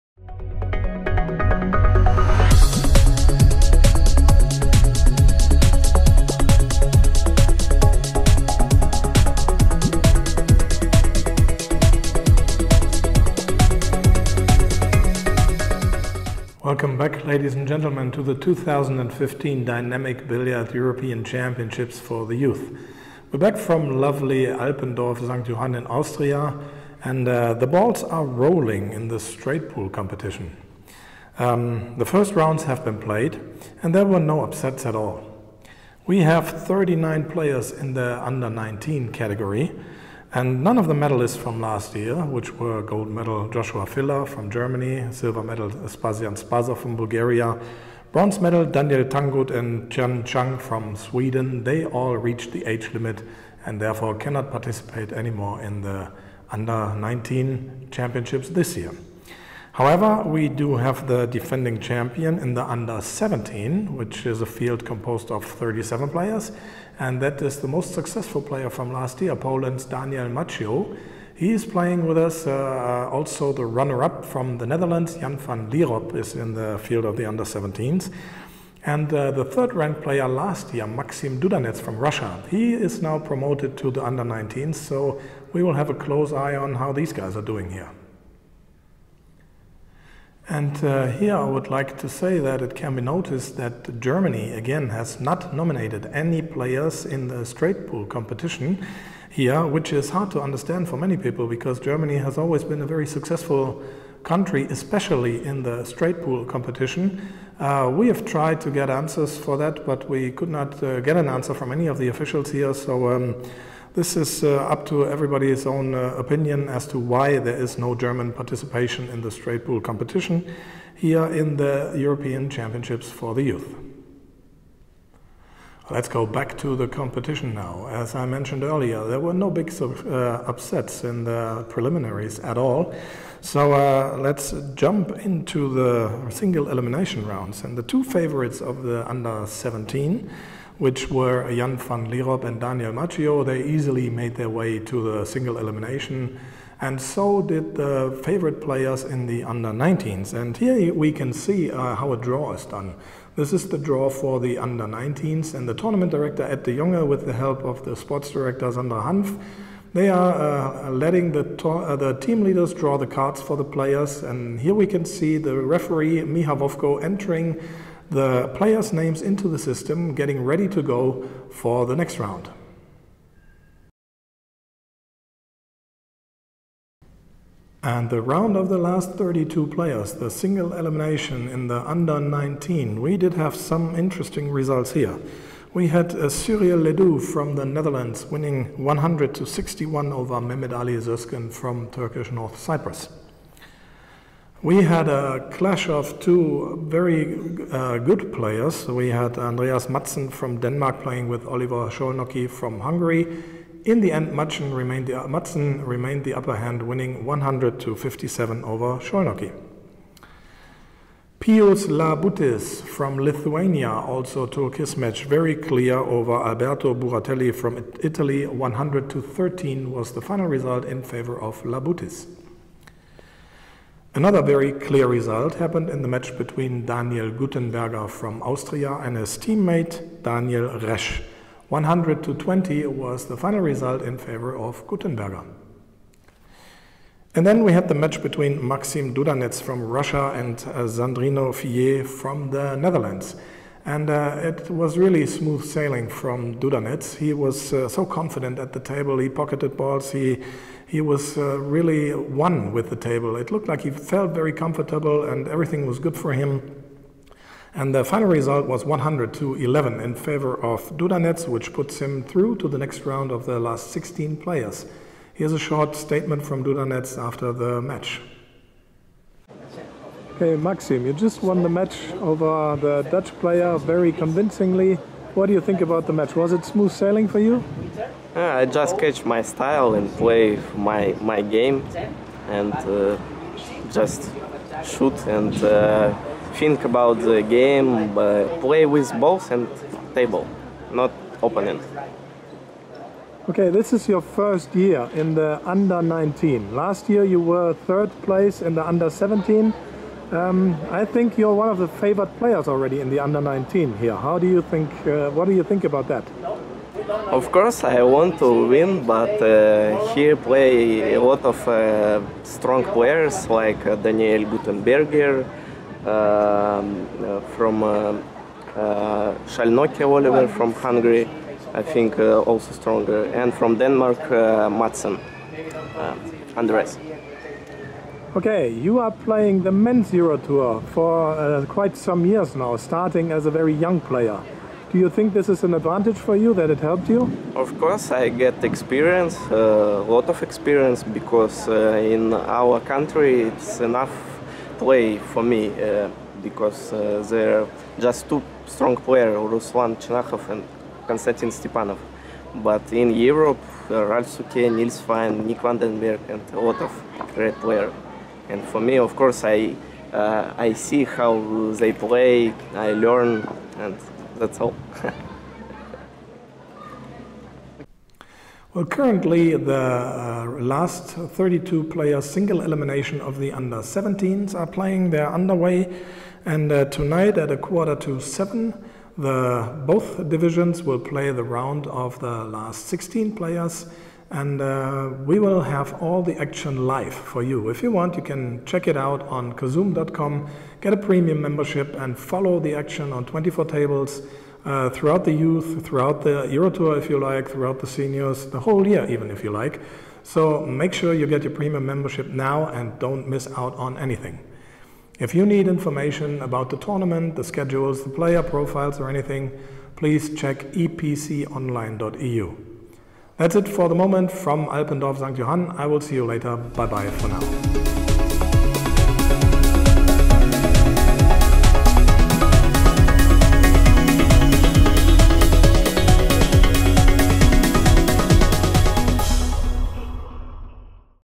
Dudanets through to last 16 in Straight Pool
At the Dynamic Billard European Championships Youth, Maksim Dudanets (RUS) won his first matches in the U19 category and made his way through to the last 16 players...